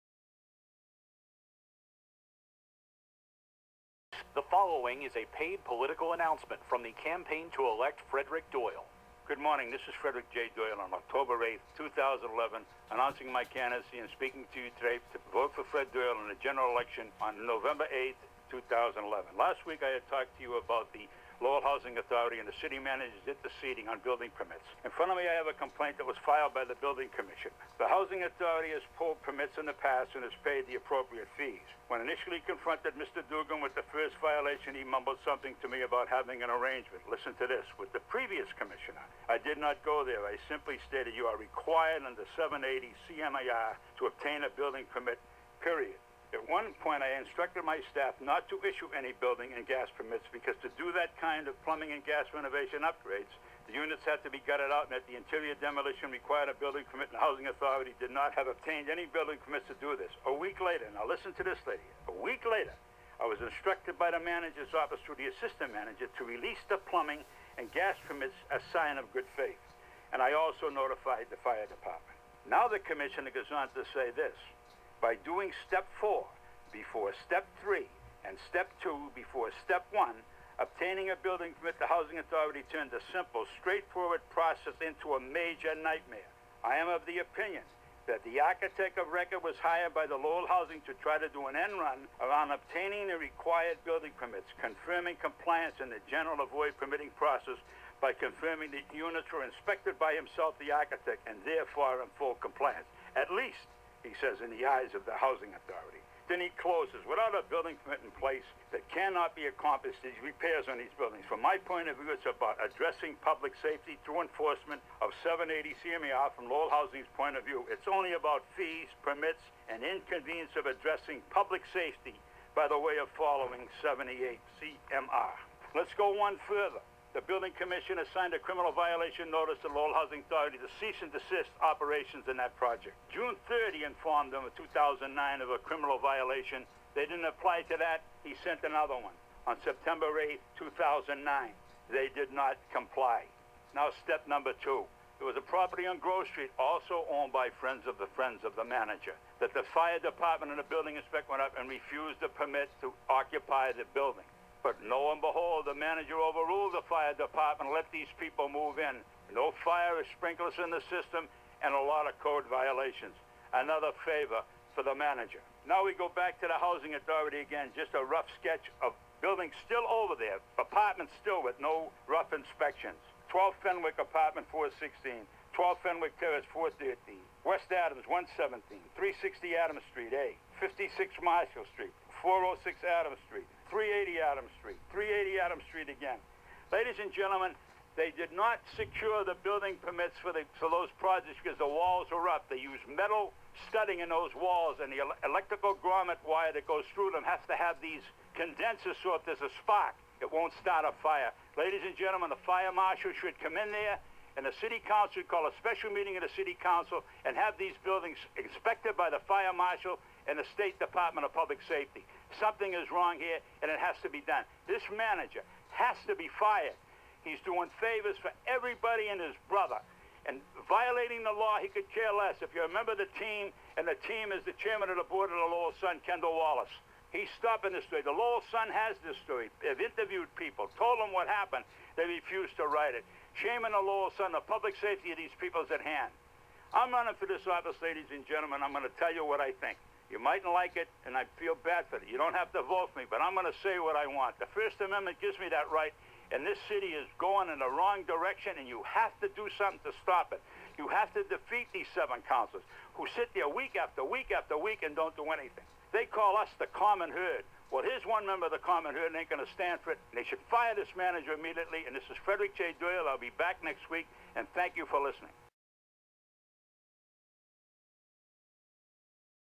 Fred Doyle October 2, 2011
Fred Doyle, candidate for Lowell City Council.  Radio commercial on October 2, 2011